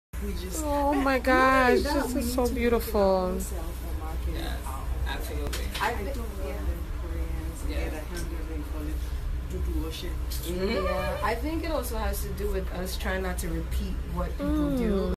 Meet Sade of Sade Skincare & Cosmetics II
Sade Skincare & Cosmetics line is a skincare and makeup line owned by Nigerian woman, Sade. The company is dedicated to keeping the memory and history of traditional African skincare alive. This is a raw footage of 3 African women meeting Sade via boakyeb.com.